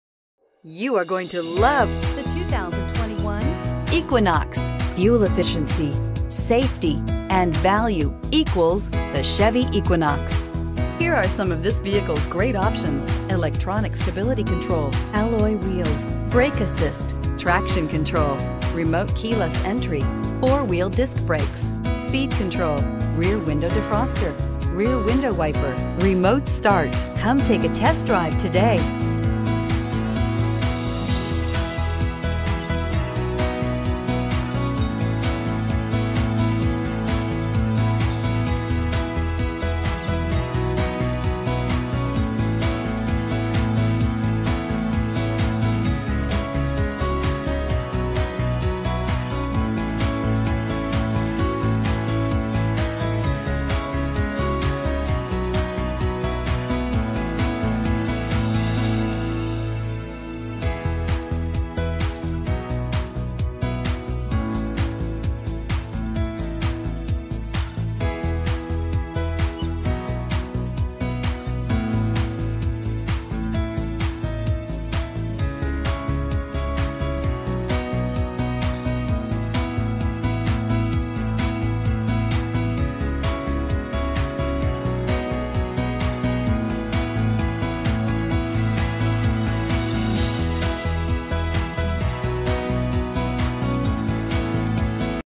2021 Chevrolet Equinox New 210312
2021 Chevrolet Equinox LT

Don McCue Chevrolet
2015 E Main St

2021 Chevrolet Equinox LT AWD. Price includes: $4000 - Chevrolet Consumer Cash Program. Exp. 03/01/2021